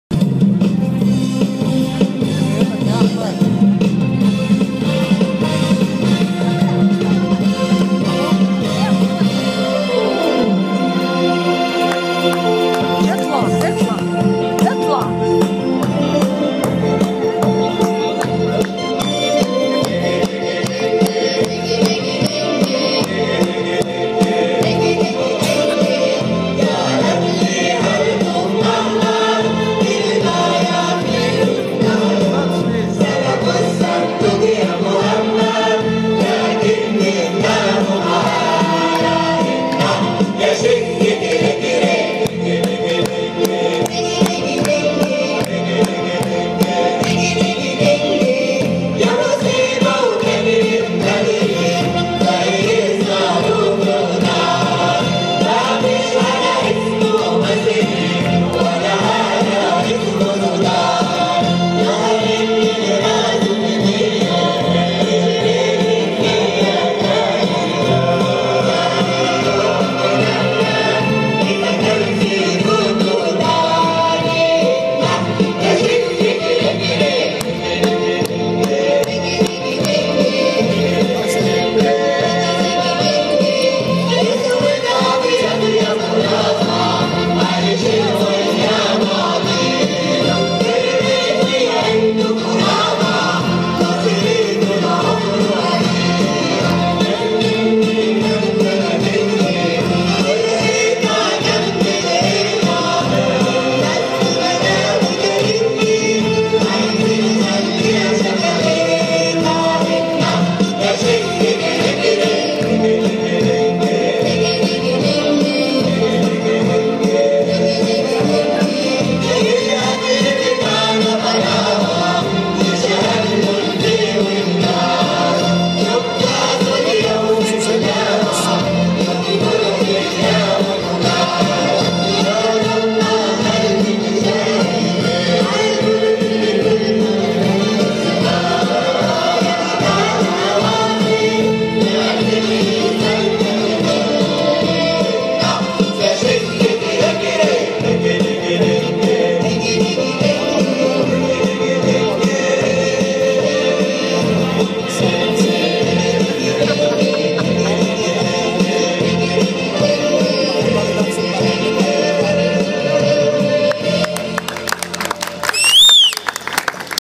حفلة العاشر من رمضان 14
حفلة العاشر من رمضان يوم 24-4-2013
لجمعية اصدقاء سيد درويش
بقيادة الاستاذ / محمد حسن درويش